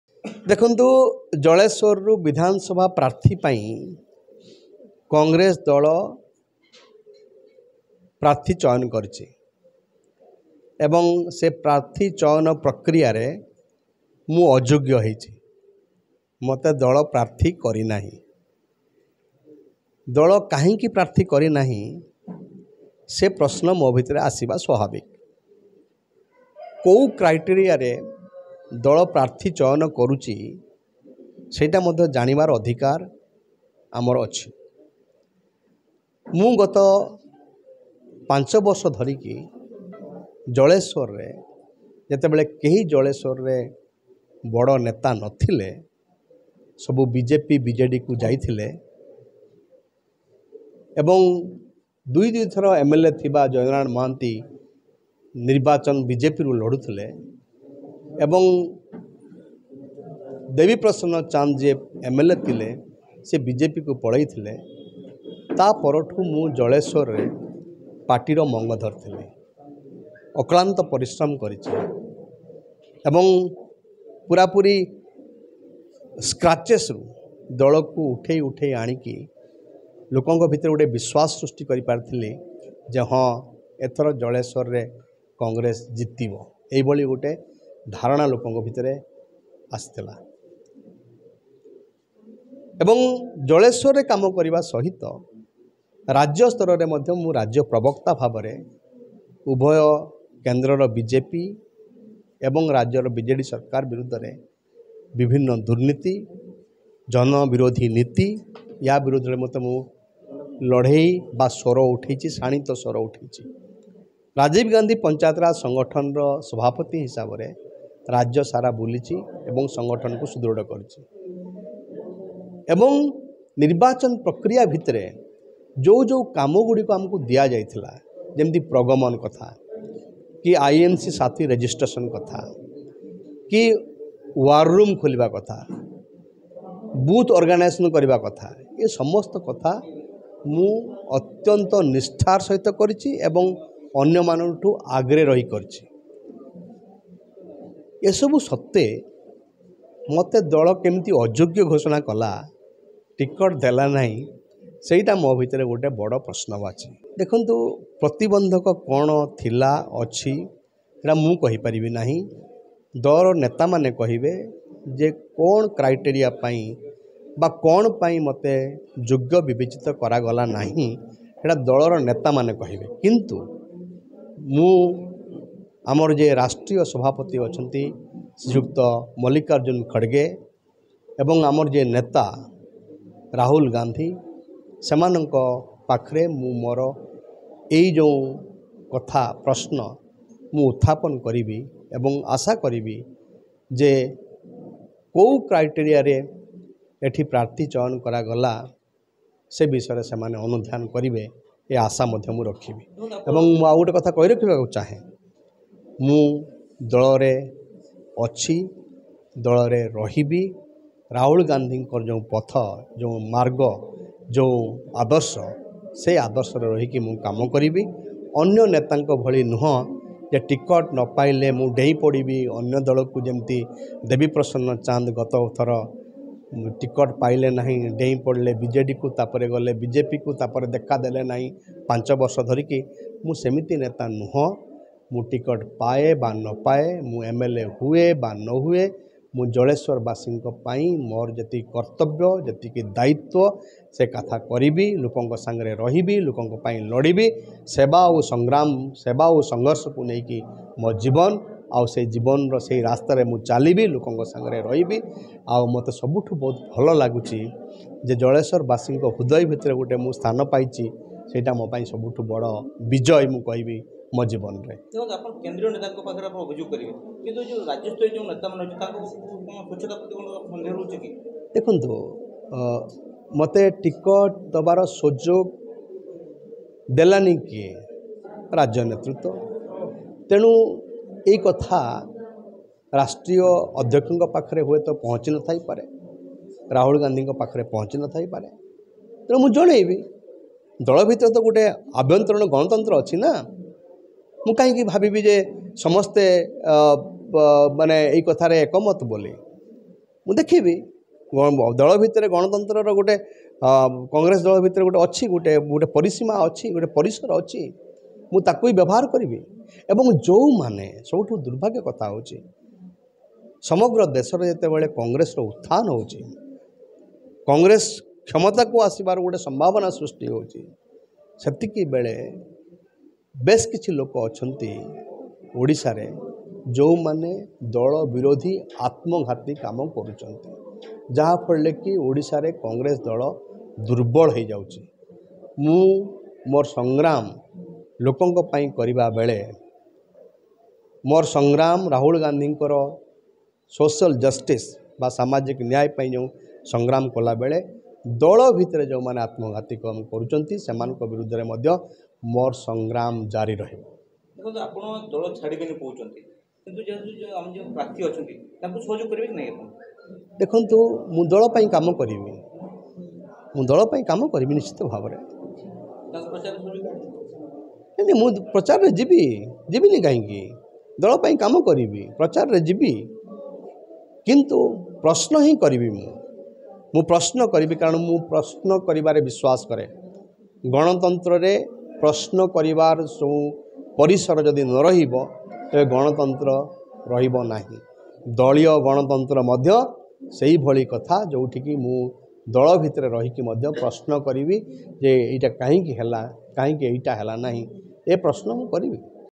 Cong Leader Sudarshan Das Questions Party's Candidate Selection Process, Asks Why Being Rejected ?
Congress leader Sudarshan Das raises important questions about the party's candidate selection process, questioning why certain candidates are being rejected